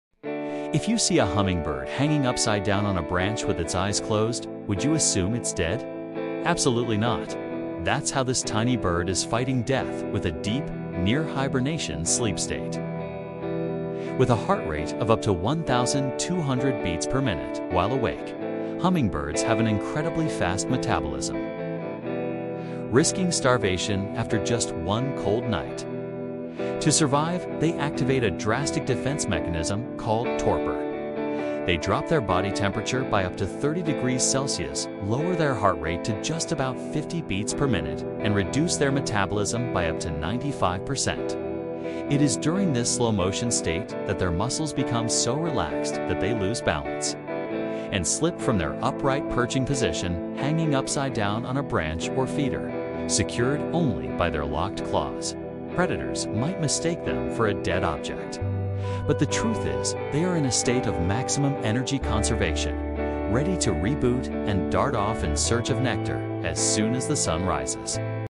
Hummingbird’s Death-Defying Sleep: Why It Hangs UPSIDE DOWN to Survive!
If you see a hummingbird hanging upside down on a branch with its eyes closed, would you assume it’s dead? ABSOLUTELY NOT! That’s how this tiny bird is fighting death with a deep, near-hibernation sleep state! Hummingbirds, with their lightning-fast metabolism (up to 1,200 BPM!), risk starving after just one cold night. To survive, they enter torpor: they drop their body temperature by up to 30°C and reduce their heart rate to about 50 BPM. It is during this extreme slowdown that they become so relaxed they often lose balance and slip into the upside-down hanging position—secured only by their amazing foot-locking mechanism. Predators mistake them for a dead insect, but they are simply in a state of maximum energy conservation, waiting for the sun to “reboot” their system.